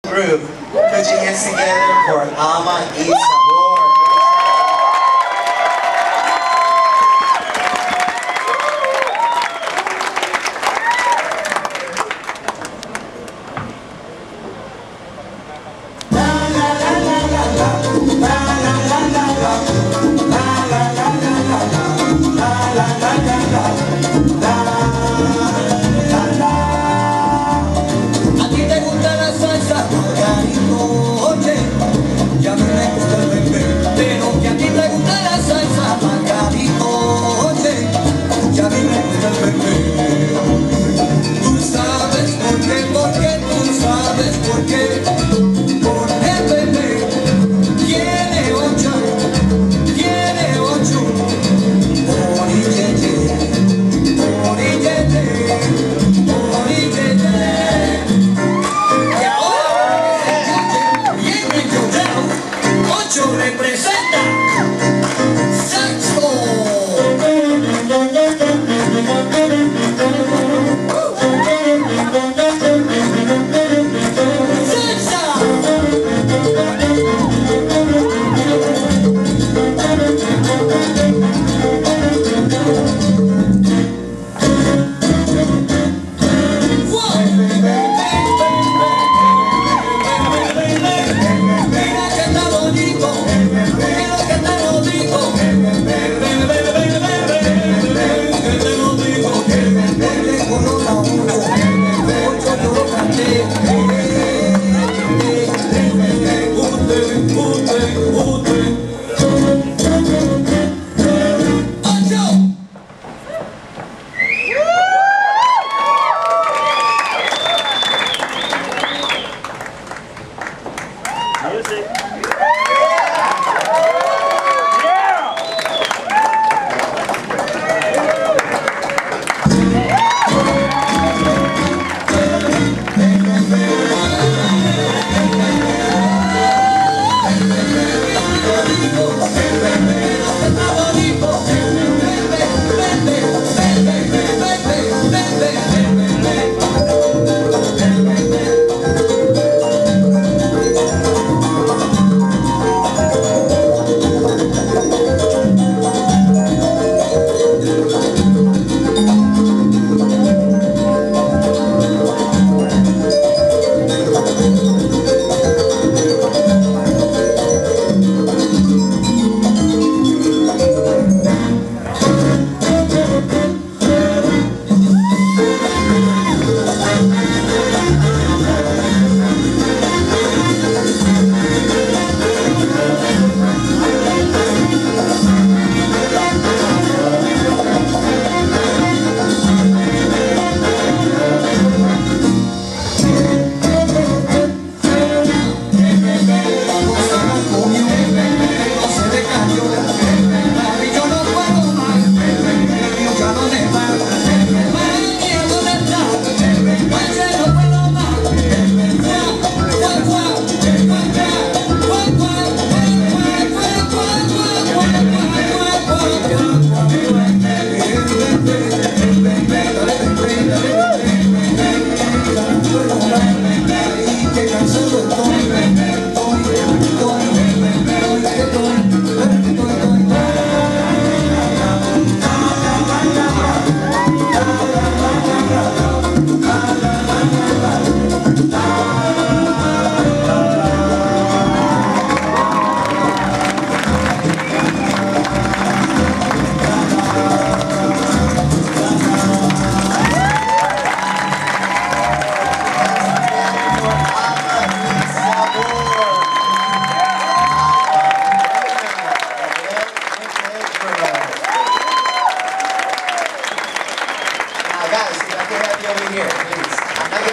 Alma y Sabor performing at Avant Garde
AyS performing at July's 31st Salsa Party in Newport Beach, California.
Alma Frey, Pedro Mendez, Mina and Abel.